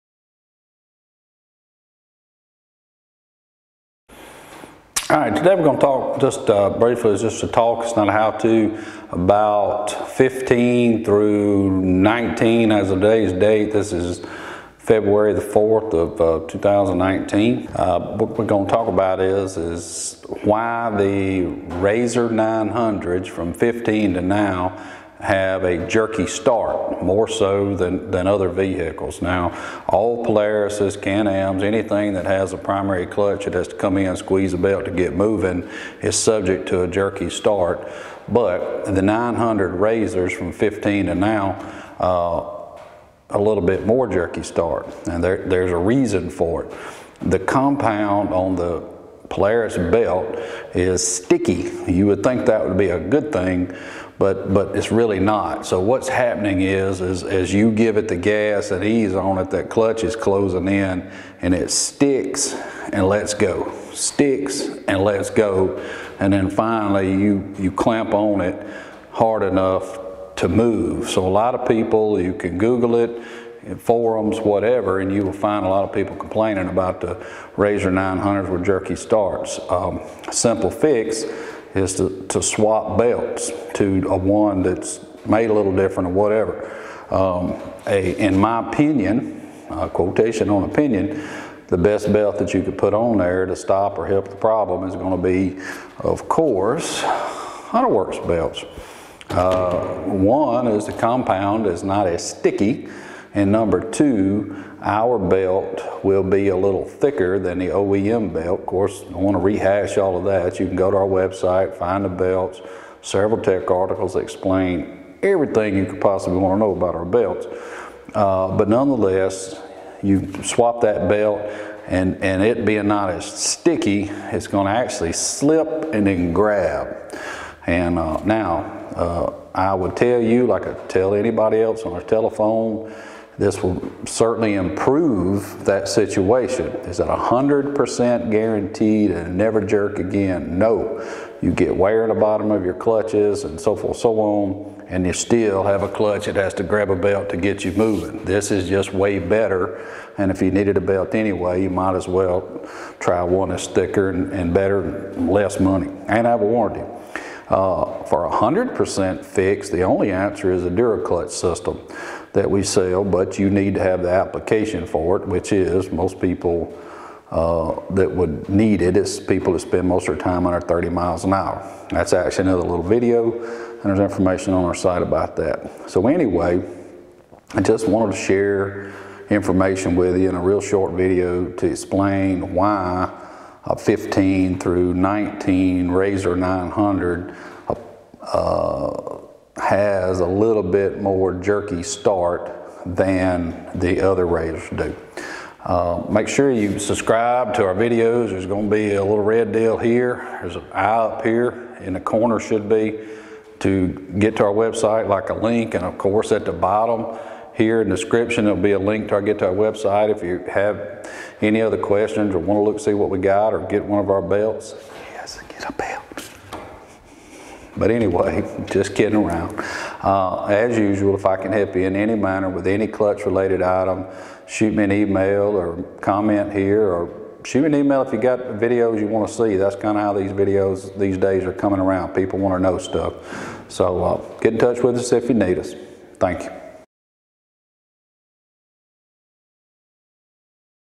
Why the 2015-23 RZR 900 has jerky starts
This video explains why the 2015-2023 Polaris RZR 900 with a factory belt that ends in 72 on the part number has a jerky start and how we fix it.